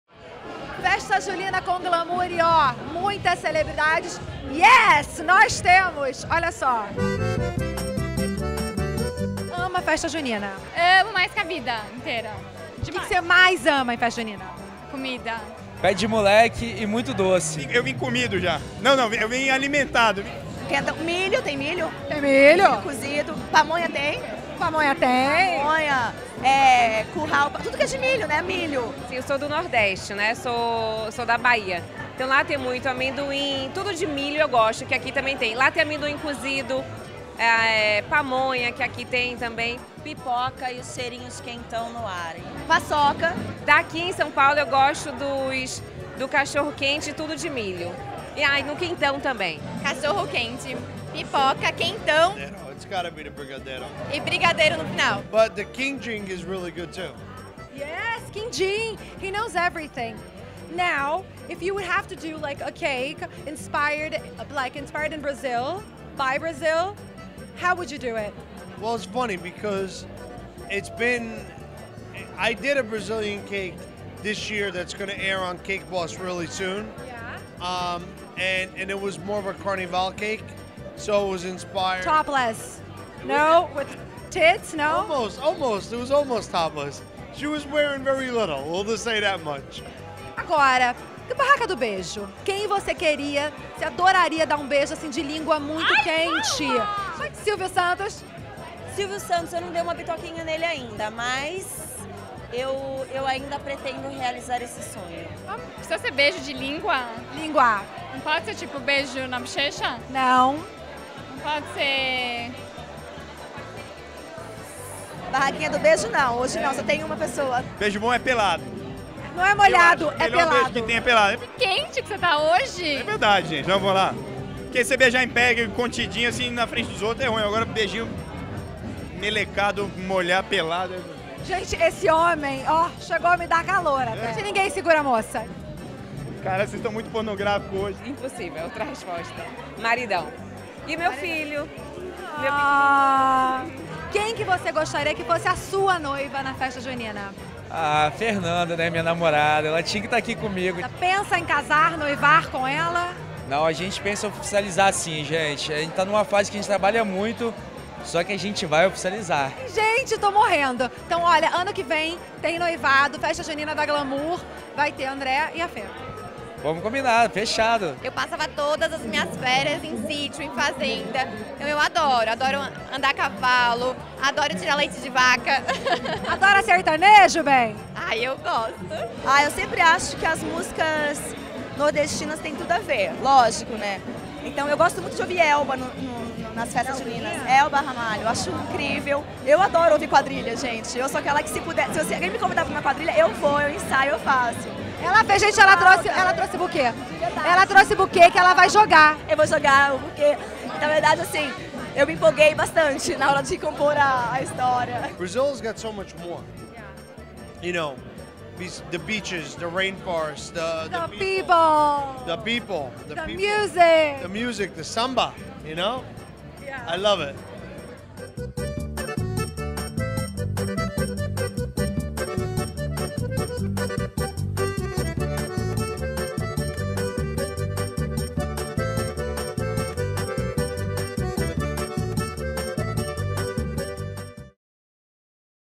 Festa Julina Fashion da Glamour 2015 | Making of
Modéstia à parte, choveu celebridade em nossa Festa Julina Fashion. Pauli Merlo, editora-sênior da revista, bateu um papo São João com Wanessa, Felipe Titto, André Martinelli e mais! Aperte o play!